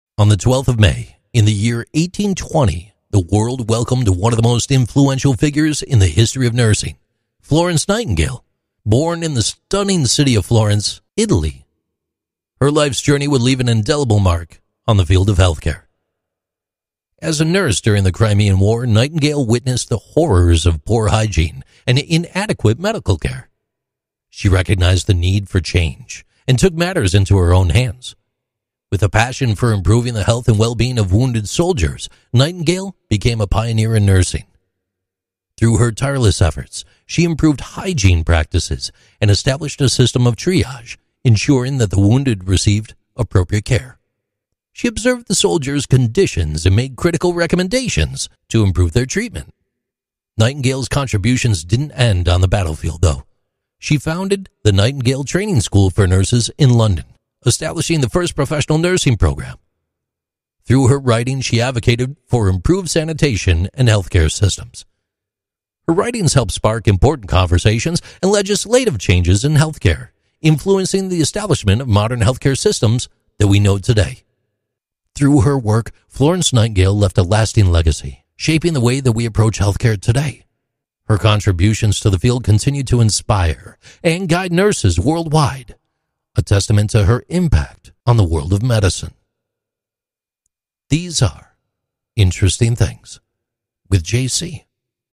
645: "Florence Nightingale"
Interesting Things with JC #645: "Florence Nightingale" - the pioneer of modern nursing, who improved healthcare during the Crimean War and founded the Nightingale Training School for Nurses was born today!

Her work still inspires us today.